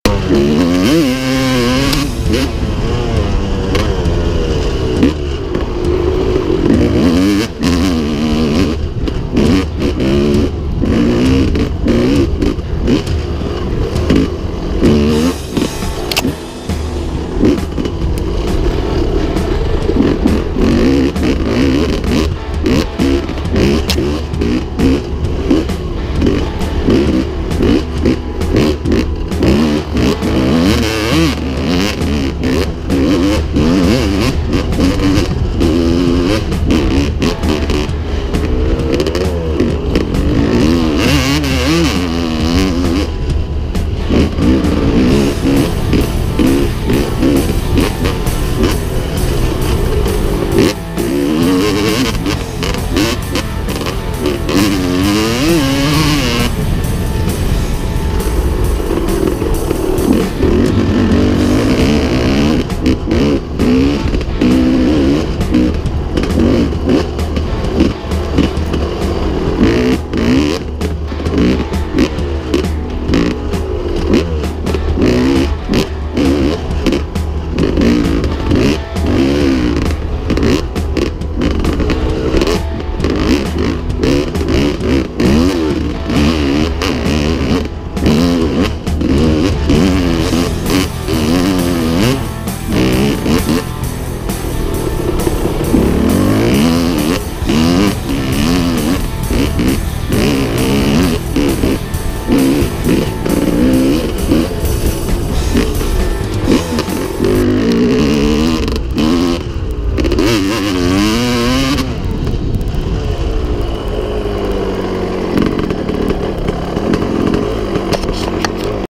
Enduro Yamaha WR250Z / 2 Hot Laps Left & Right & Tree Turns - 18
This is a short fun track uphills and downhills, over logs and pins even some stones. Sometimes the part also is with an angle, like how to say ride on the slope through the forrest :)